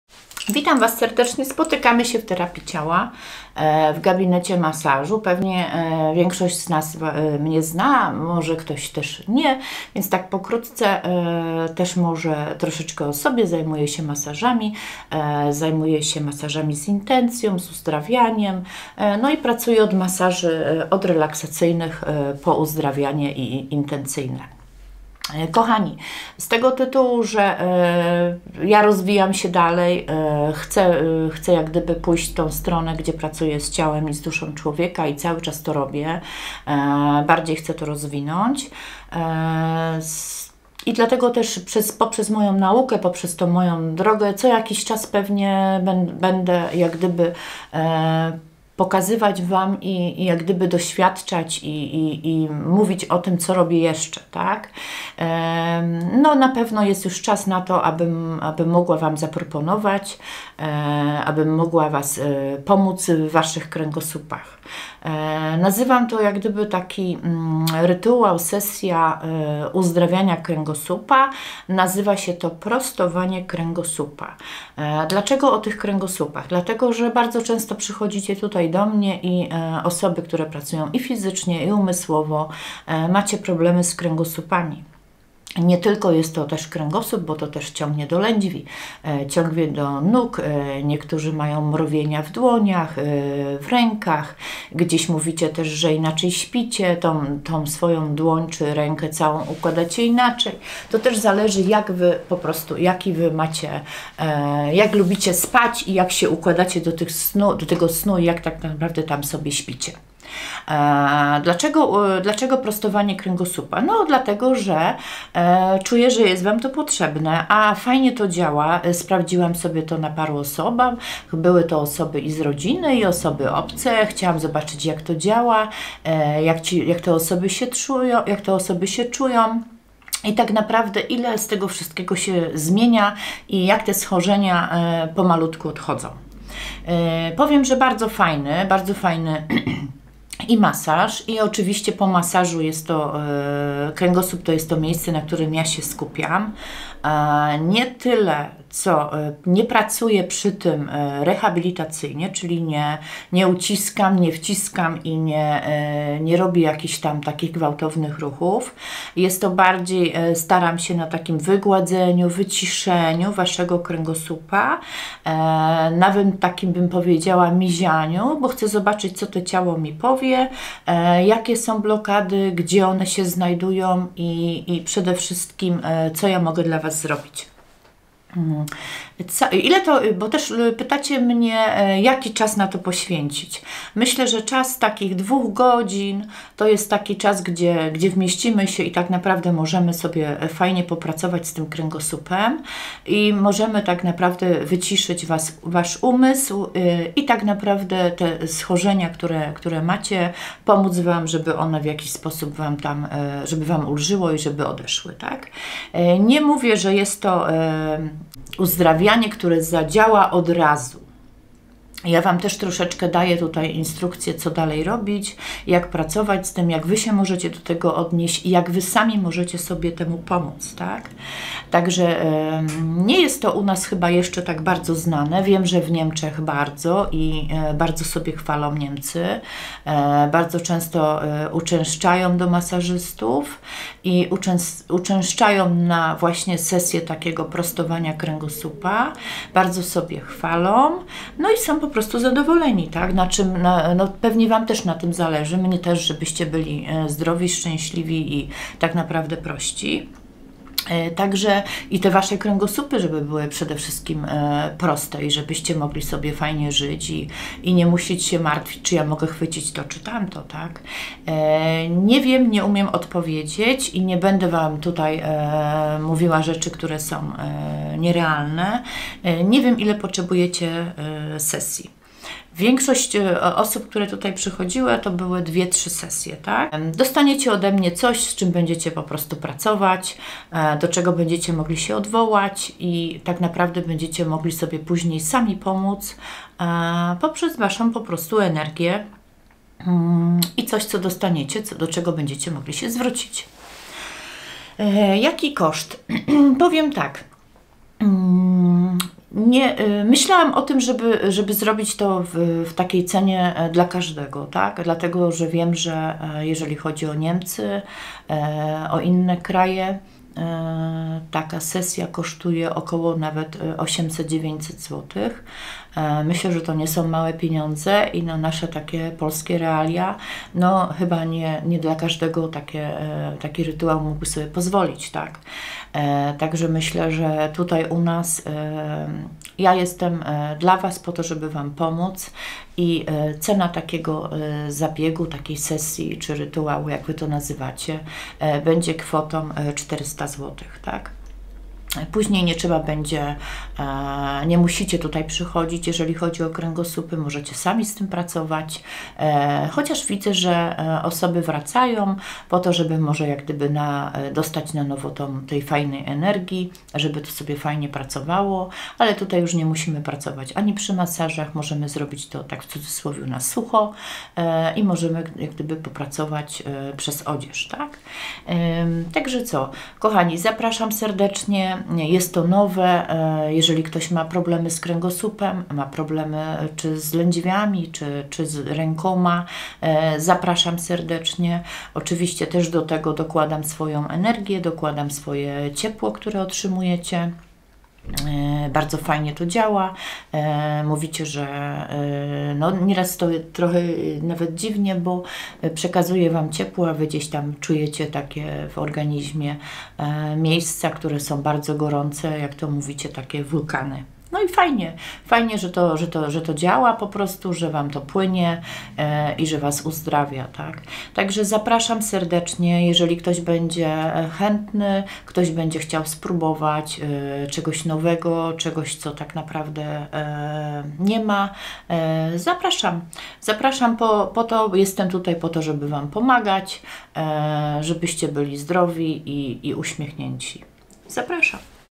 🙏. PROSTOWANIE KRĘGOSŁUPA 💚. TERAPIA CIAŁA SOKOŁA 5 POZNAŃ Barbara Ignac-Gorońska
Witajcie moi drodzy ❤️.   w tym filmie tłumaczę jak wygląda prostowanie kręgosłupa na poziomie energii z która pracuję , jeśli macie jakieś pytania to proszę piszcie lub  dzwońcie , odpowiem  na każde z nich w wolnej chwili , pozdrawiam Barbara Ignac-Gorońska